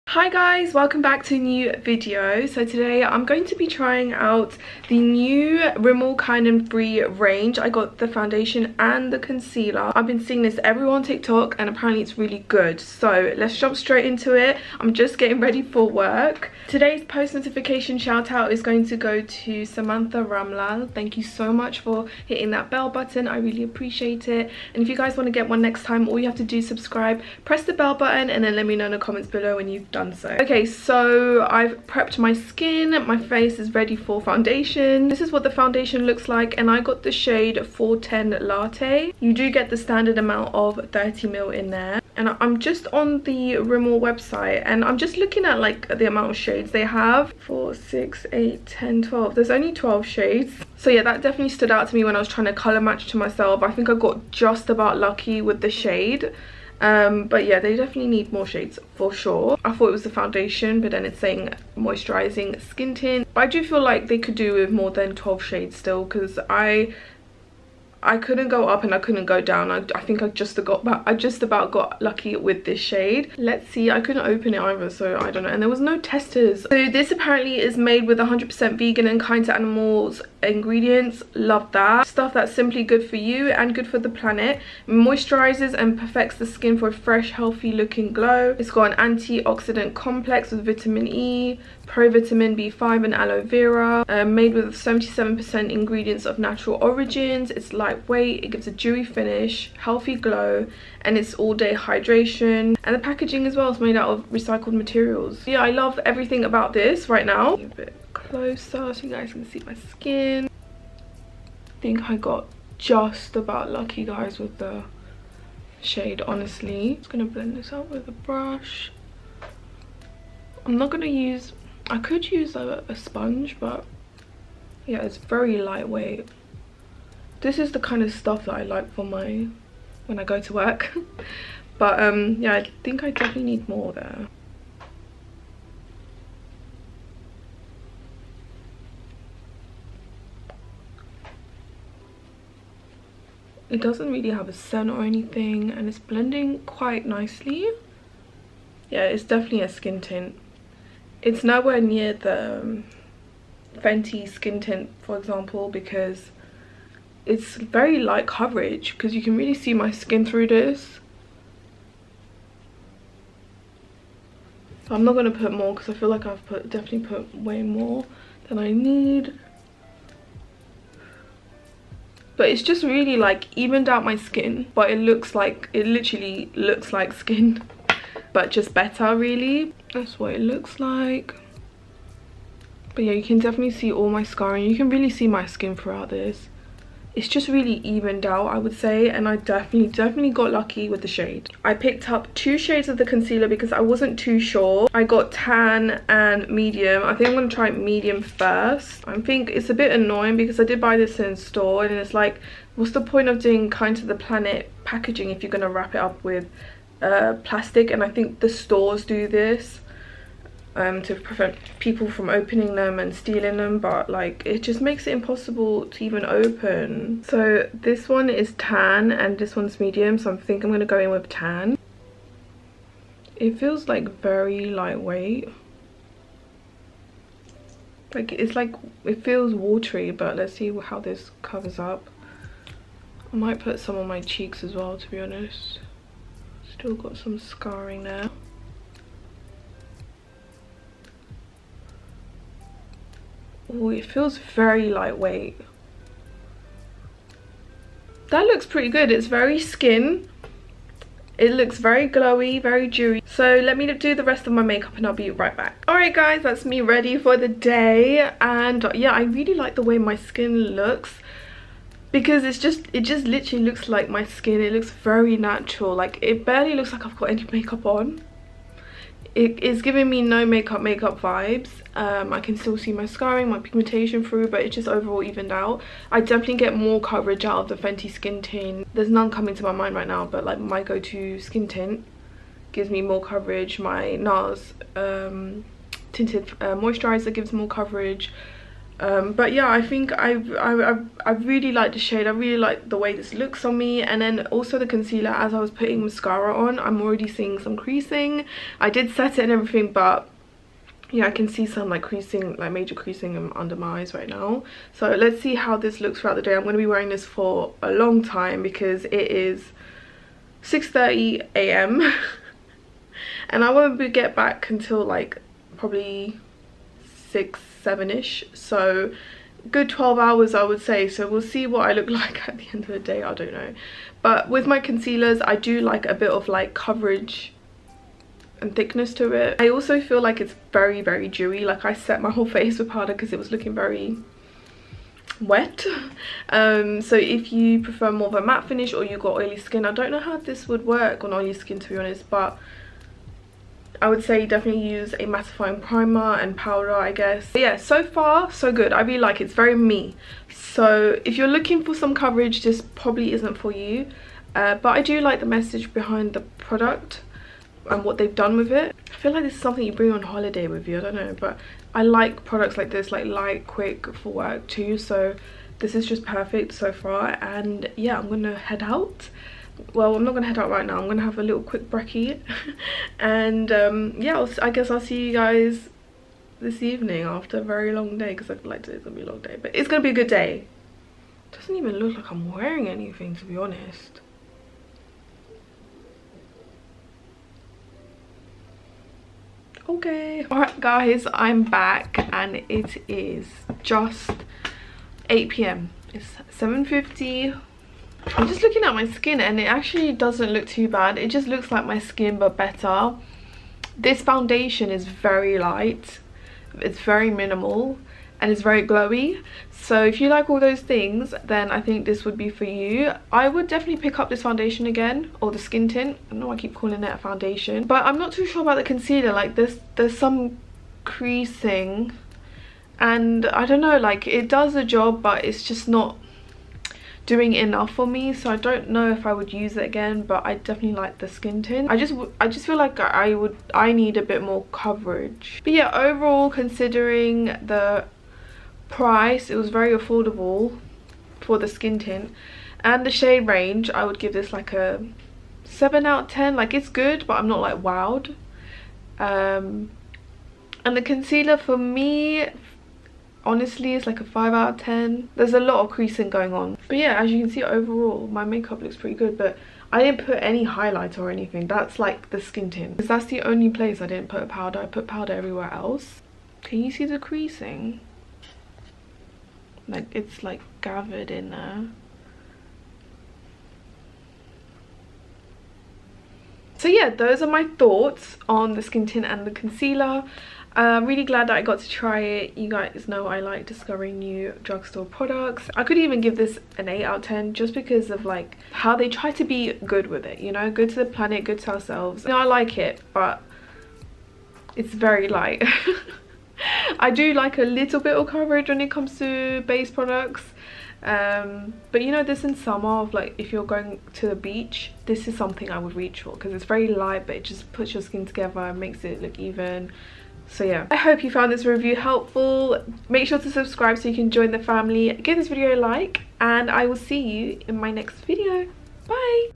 TESTING NEW RIMMEL KIND & FREE SKIN TINT + CONCEALER | 13 HR WEAR TEST
Rimmel London Kind & Free Moisturising Skin Tint Foundation 410 Latte